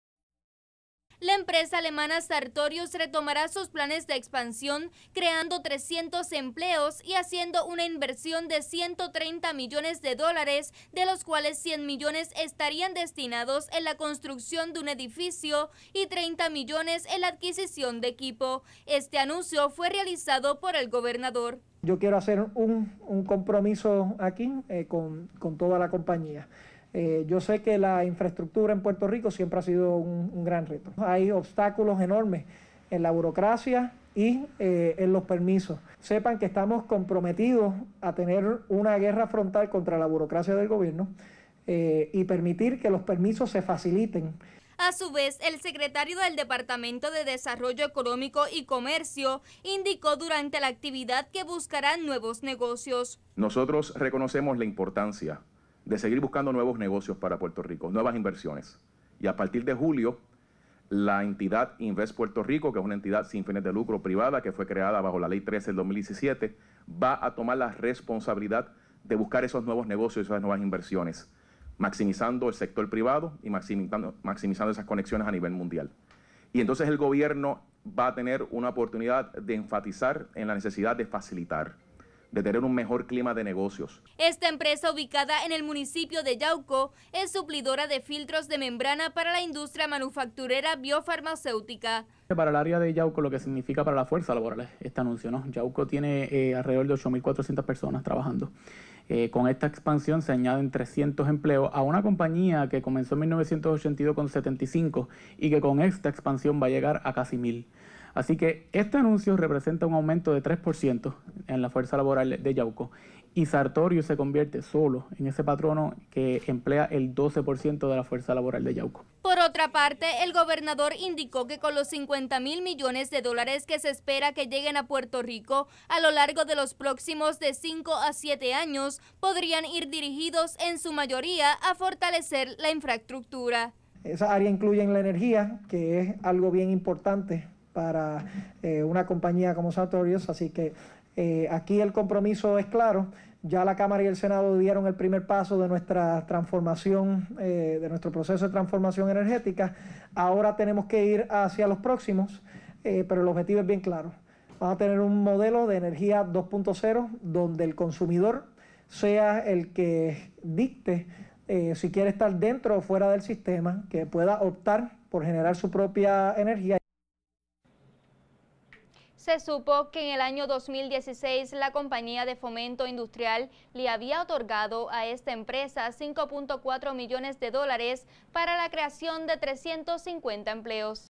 Gobernador anuncia la creación de empleos en Yauco
El gobernador de Puerto Rico, Ricardo Rosselló, anunció la expansión de la empresa alemana Sartorius, ubicada en el municipio de Yauco. Según se indicó, esto generaría 300 nuevos empleos.